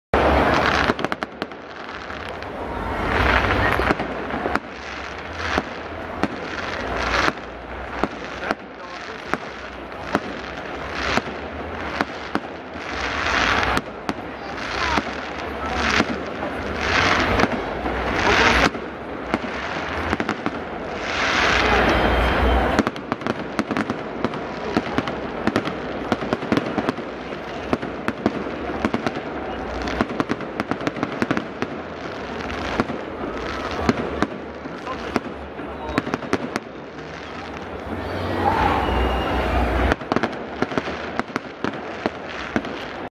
Киров. День города-2013. Праздничный фейерверк
Праздничный фейерверк в День города-2013 в Кирове на Театральной площади.